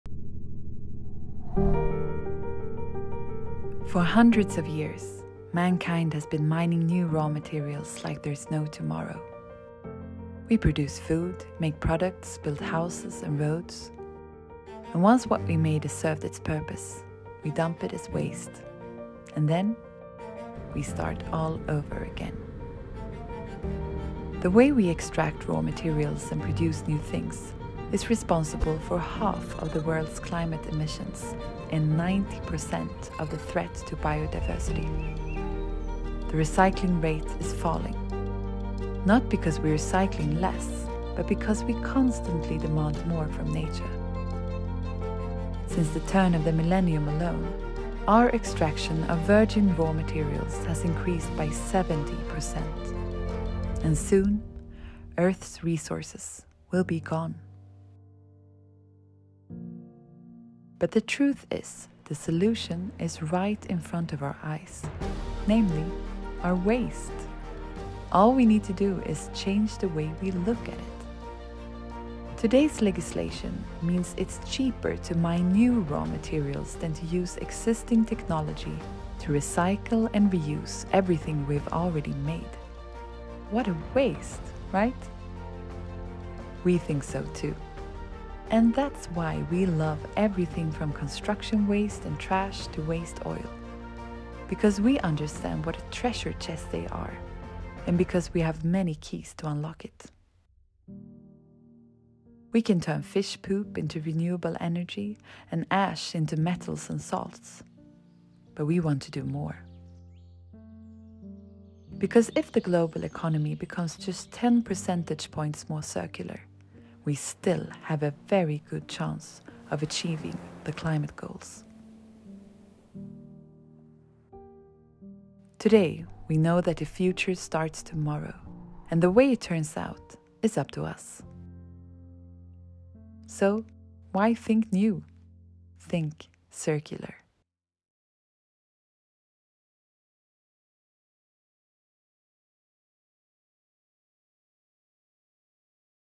What a waste!
Vårt avfall er en skattekiste fylt med råvarer som kan gjenbrukes, men som oftest ikke blir det. Ragn-Sells sin kampanje med tittelen «What a waste!», som har som mål å endre synet på avfall og drive omstillingen mot et sirkulært samfunn.